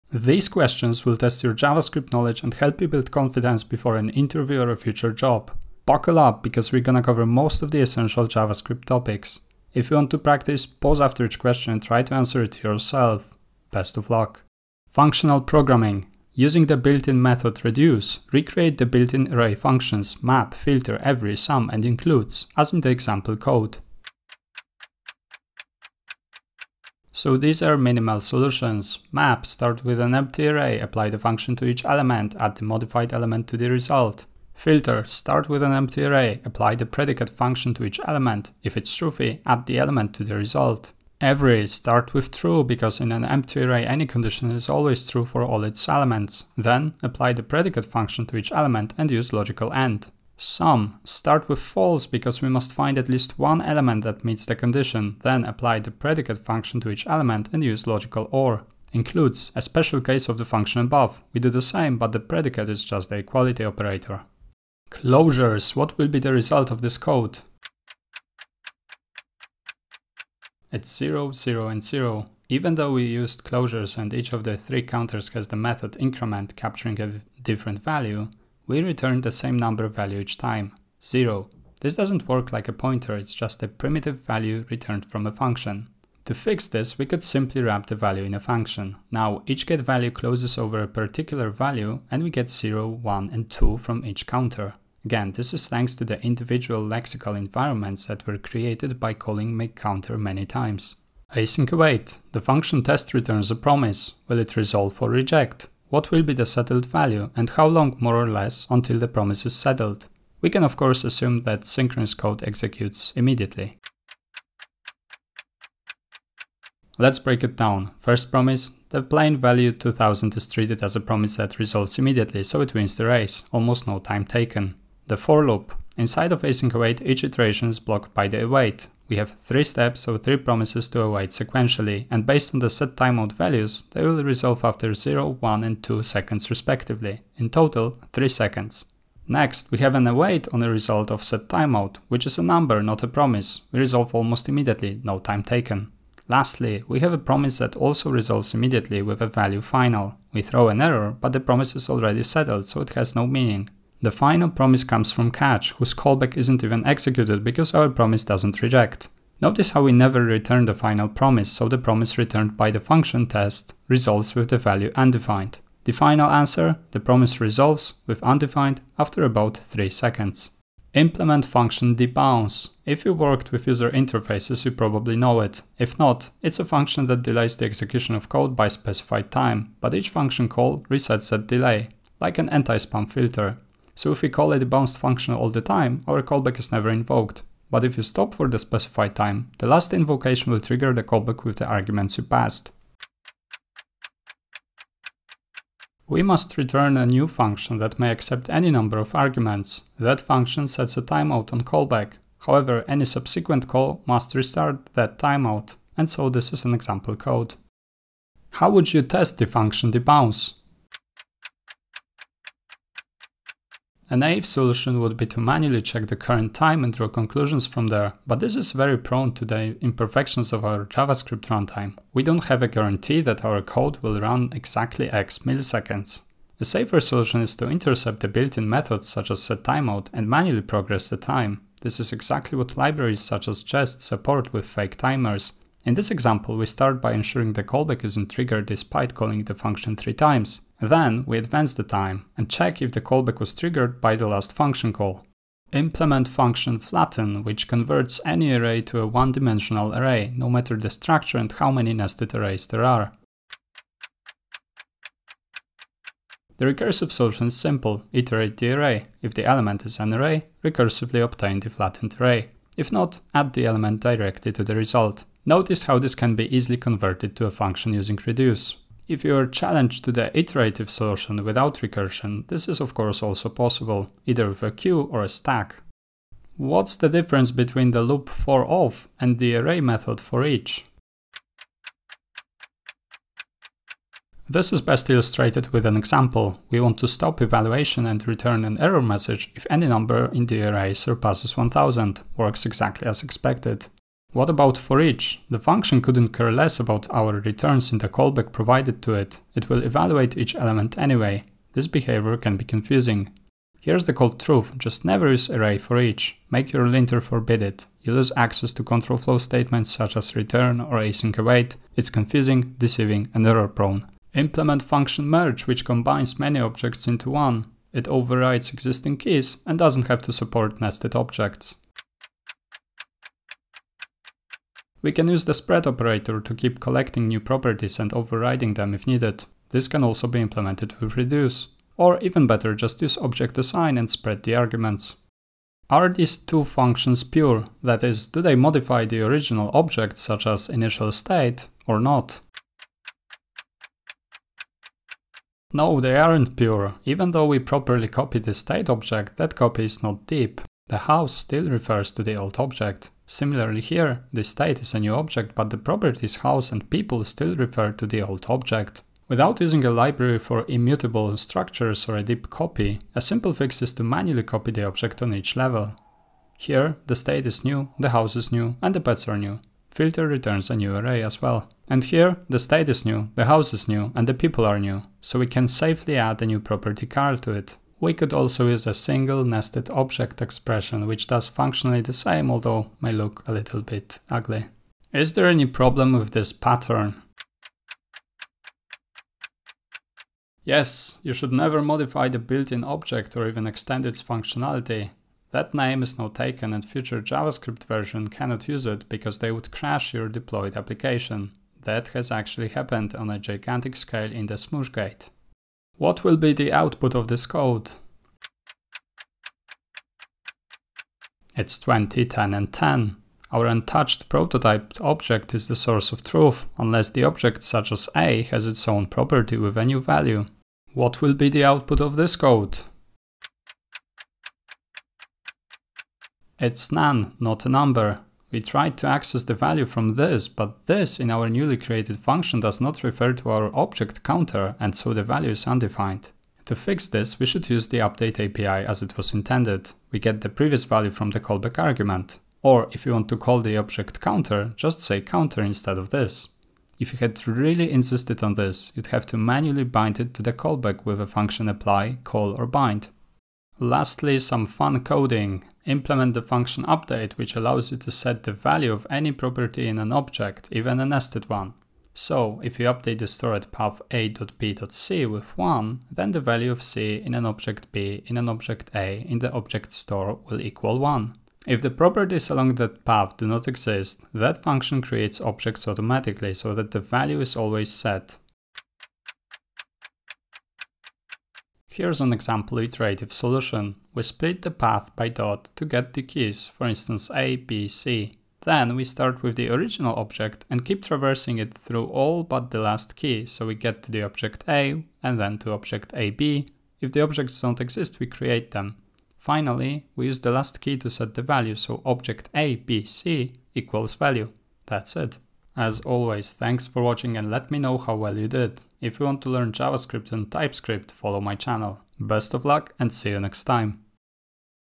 How Good Is Your JavaScript? (Essential Topics)
13 JavaScript interview puzzles - theoretical questions and practical coding tasks. Essential JavaScript topics: arrays, objects, functional programming, higher-order functions, pure functions, immutability, closures, asynchronous code, promises, async-await, callbacks, testing, loops, classes, prototypes, references, and the keyword "this".

0:00 Introduction
0:15 Q1 Array methods with reduce
1:04 Q2 Closures
1:41 Q3 Async/await
2:50 Q4 Debounce
3:28 Q5 Test debounce
4:05 Q6 Flatten array
4:36 Q7 Loop for..of vs Array.forEach
5:13 Q8 Merge objects
5:35 Q9 Pure state updates
6:25 Q10 Extending built-in prototype
6:45 Q11 Prototype inheritance
7:00 Q12 "this" context
7:33 Q13 Update object at path